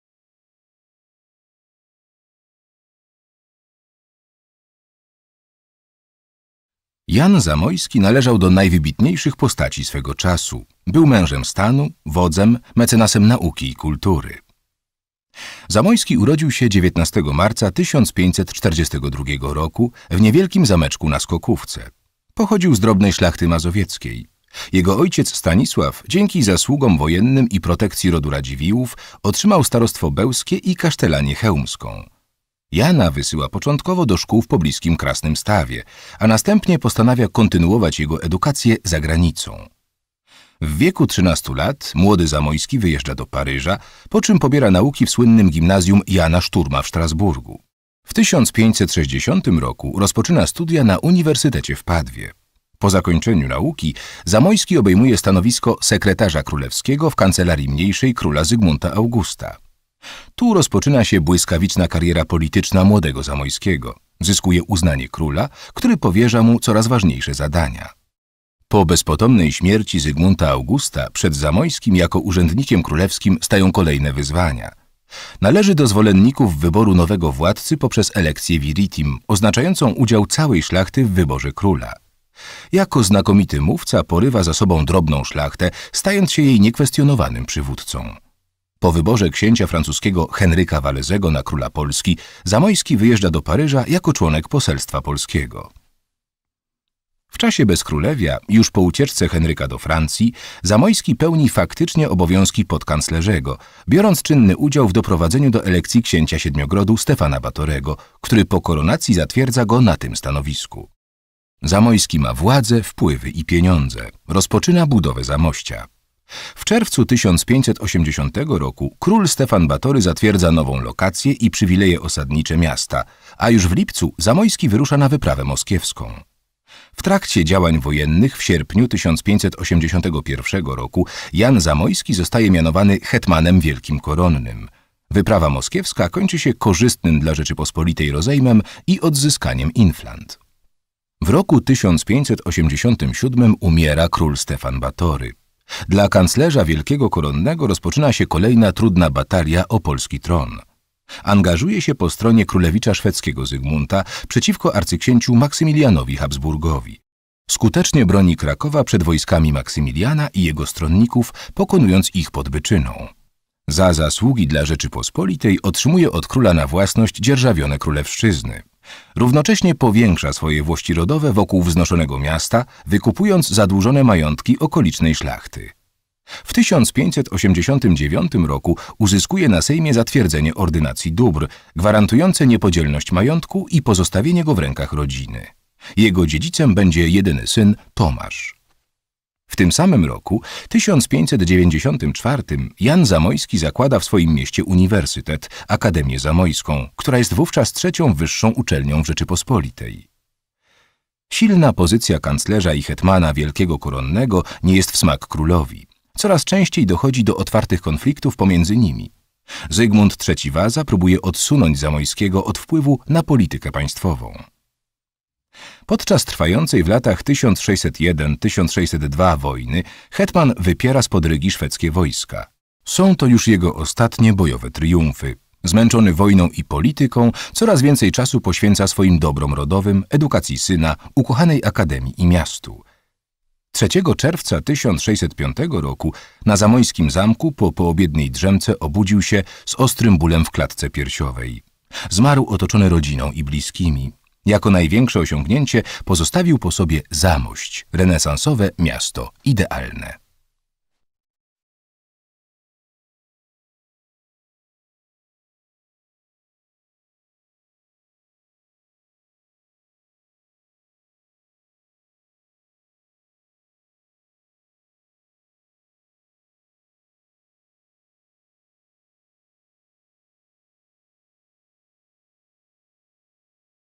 PASJONACI HISTORII : Jan Sariusz Zamoyski (Jan Zamojski)
Jan Sariusz Zamoyski (Jan Zamojski) herbu Jelita (ur. 19 marca 1542, zm. 3 czerwca 1605) – sekretarz królewski od 1565, podkanclerzy koronny od 1576, kanclerz wielki koronny od 1578 i hetman wielki koronny Rzeczypospolitej Obojga Narodów od roku 1581. Generalny starosta krakowski w latach 1580–1585, starosta bełski, malborski, grodecki, jaworowski, krzeszowski, tykociński i dorpacki, starosta międzyrzecki w 1580 roku, starosta knyszyński w 1574 roku[3]. Doradca króla Zygmunta II Augusta i Stefana Batorego. Główny przeciwnik sukcesora po Batorym, Zygmunta III Wazy. Humanista-mecenas, filolog i mówca, magnat.

Jako senator brał udział w sejmach: 1601, 1603 i 1605 roku.